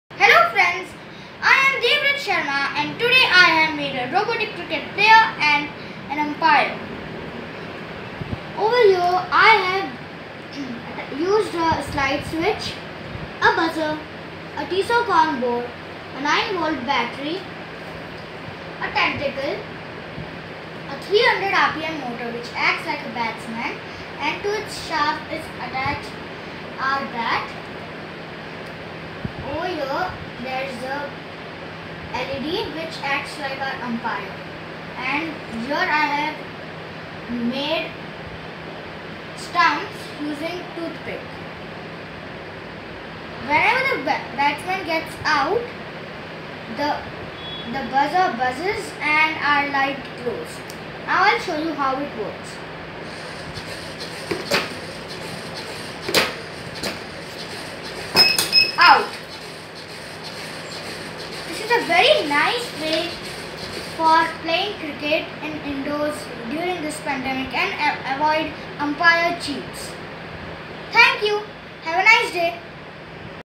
Cricket player - Devavrat Sharma
With the pandemic forcing people to stay indoors, playing indoor games was good way to pass time. So, our gaming champ Devavrat designed his own cricket playing robot to enjoy with everyone. He even added an umpire to check if a player is out or not to prevent cheating.